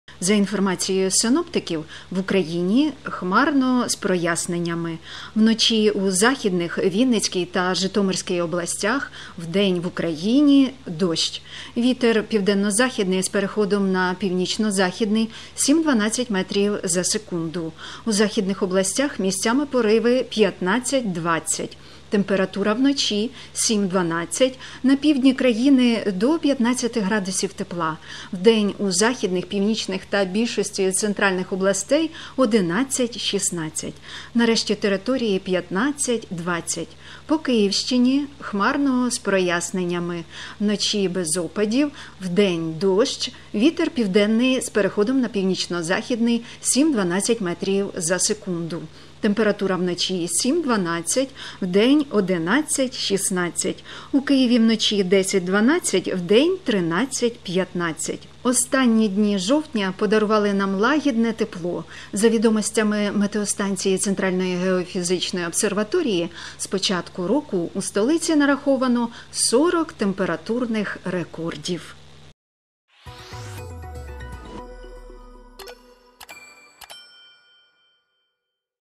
☀️Прогноз погоди на 1 листопада
За інформацією синоптиків, в Україні: хмарно з проясненнями. Вночі у західних, Вінницькій та Житомирській областях, вдень в Україні дощ. Вітер південний з переходом на північно-західний, 7-12 м/с, у західних областях місцями пориви 15-20 м/с.
Температура вночі 7-12°, на півдні країни до 15°, вдень у західних, північних та більшості центральних областей 11-16°, на решті території 15-20°.
По Київщині: хмарно з проясненнями. Вночі без опадів, вдень дощ. Вітер південний з переходом на північно-західний, 7-12 м/с. Температура вночі 7-12°, вдень 11-16°; у Києві вночі 10-12°, вдень 13-15°.
Останні дні жовтня подарували нам лагідне тепло. За відомостями метеостанції Центральної геофізичної обсерваторії: з початку року у столиці нараховано 40 температурних рекордів.